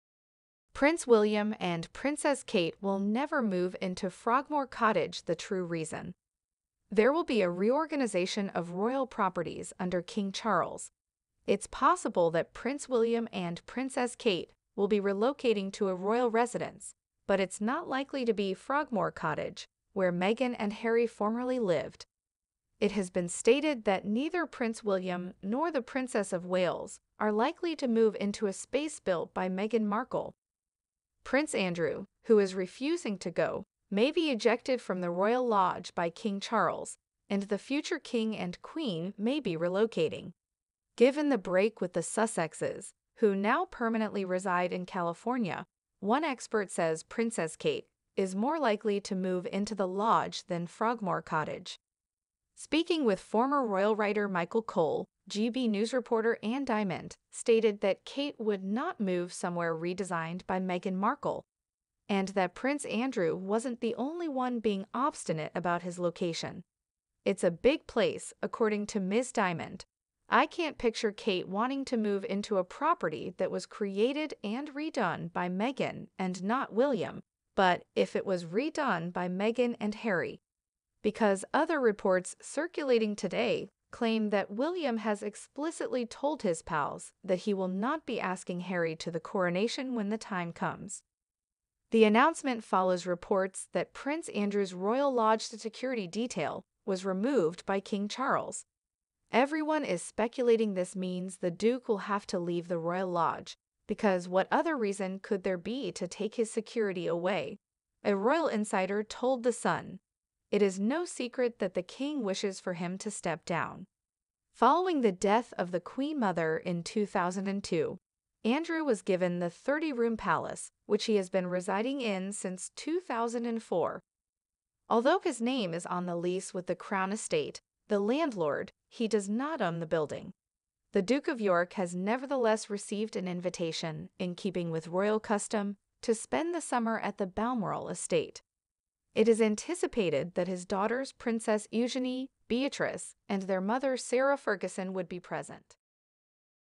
ROYALS IN SHOCK! Prince William & Princess Kate will never move into Frogmore Cottage the true reaso
Prince William and Princess Kate will never move into Frogmore Cottage the true reason

There will be a reorganisation of royal properties under King Charles. It's possible that Prince William and Princess Kate will be relocating to a royal residence, but it's not likely to be Frogmore Cottage, where Meghan and Harry formerly lived. It has been stated that neither Prince William nor the Princess of Wales are likely to move into a space built by Meghan Markle. Prince Andrew, who is refusing to go, may be ejected from the Royal Lodge by King Charles, and the future King and Queen may be relocating. Given the break with the Sussexes, who now permanently reside in California, one expert says Princess Kate is more likely to move into the Lodge than Frogmore Cottage. Speaking with former royal writer Michael Cole, GB News reporter Anne Diamond stated that Kate would "not move somewhere redesigned by Meghan Markle" and that Prince Andrew wasn't the only one being obstinate about his location.